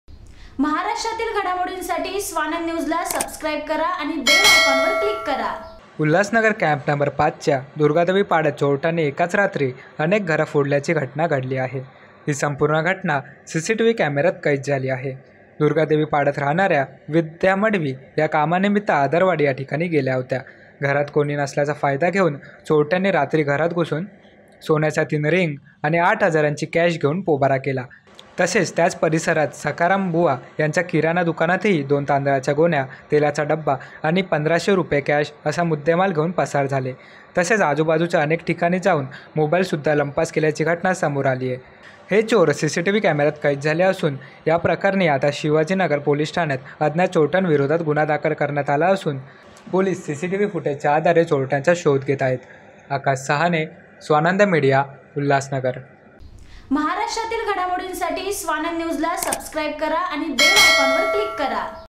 Crime | चोरट्यांचा हैदोस | अनेक घरात चोरी | चोर सीसीटीव्हीत कैद | दुर्गापाडा ! उल्हासनगर
SEND ME STUFF:

Near B.K.No.1416,
Maratha section - 32,
Ulhasnagar - 421004,
Dist. : Thane,
Maharashtra,
India